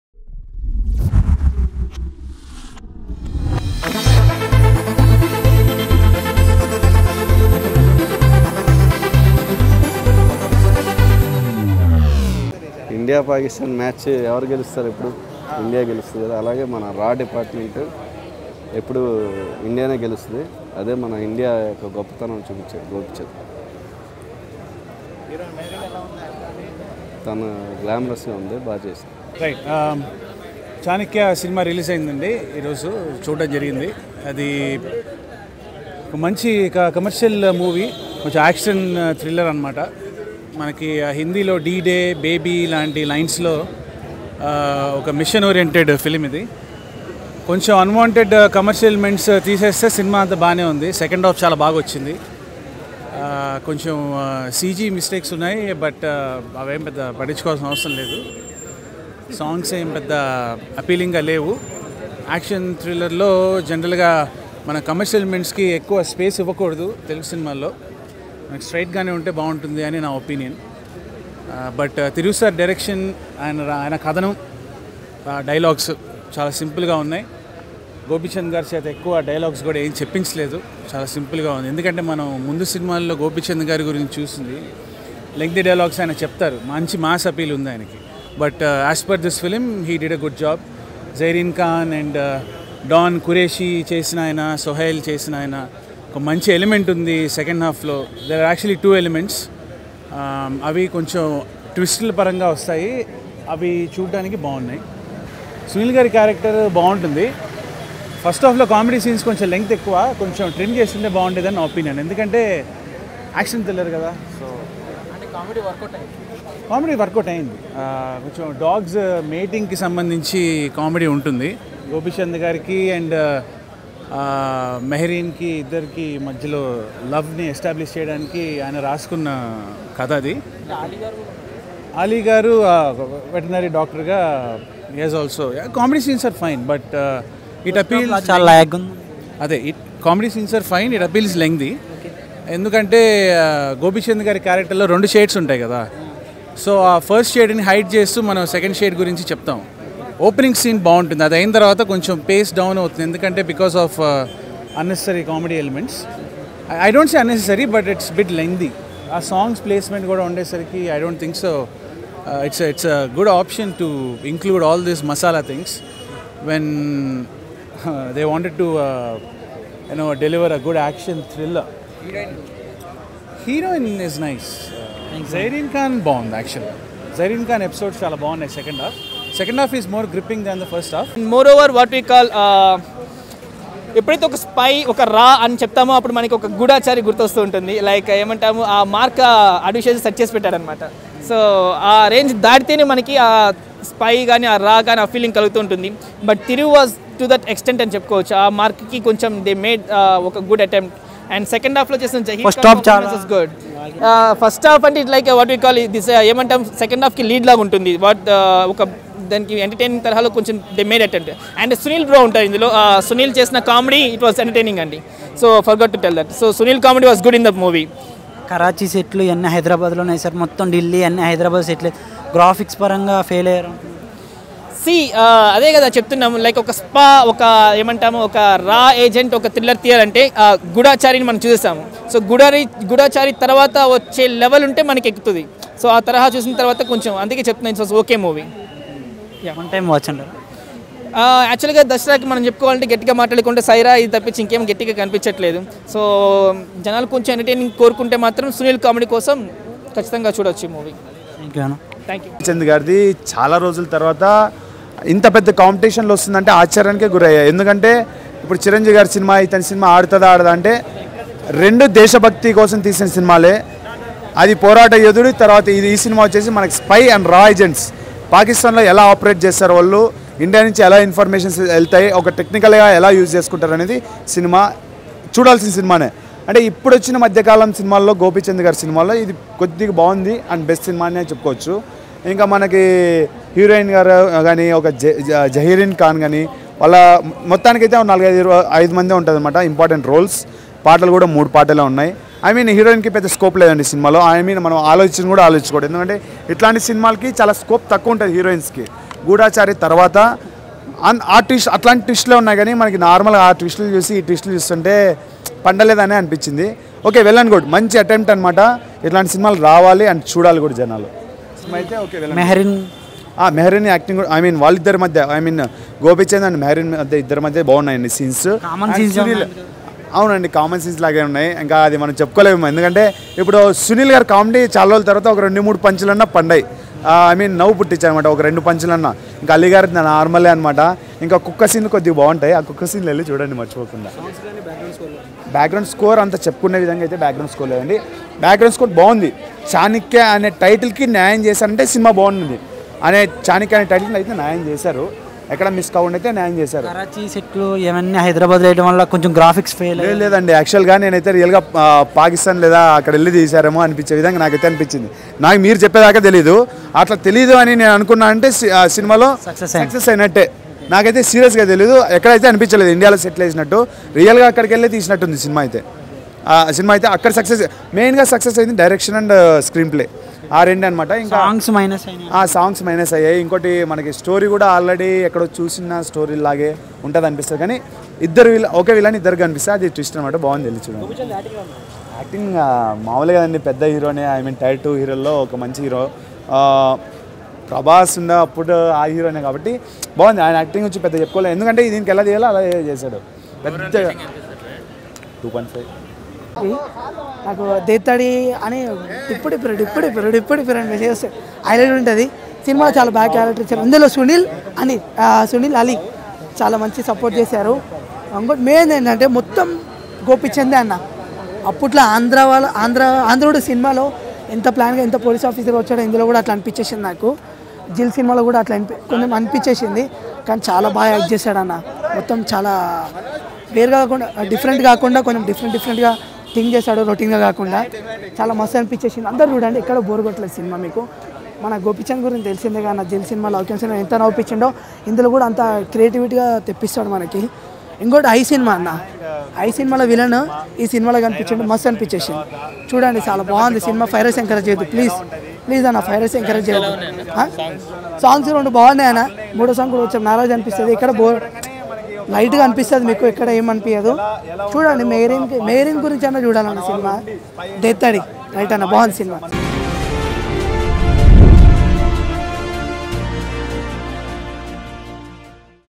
3వ రోజు ఈ మార్పు ఊహించలేదుChanakya Movie Public Talk | Public Excellent Words On Gopichand | Mehreen
Chanakya Movie Public Talk | Public Excellent Words On Gopichand | Mehreen Pirzada | Raatnam Media